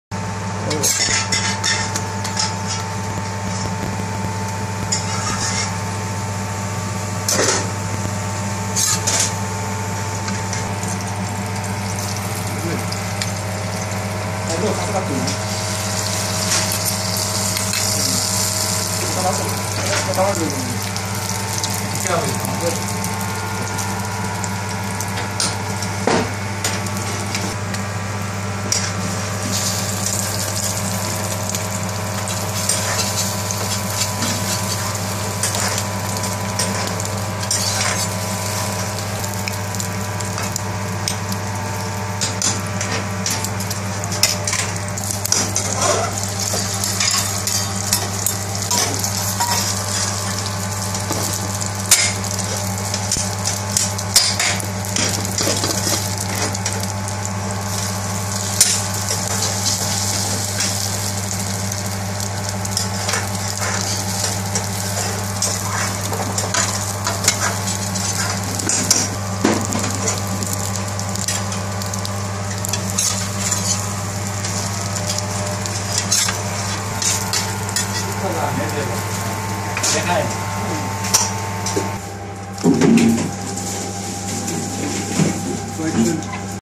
Egg Paratha -Egg paratha fluffy bread - made from chapati ata -Indian Restaurant Cooking
Egg paratha fluffy bread made from chapati ata cover with scrambled egg, was made for staff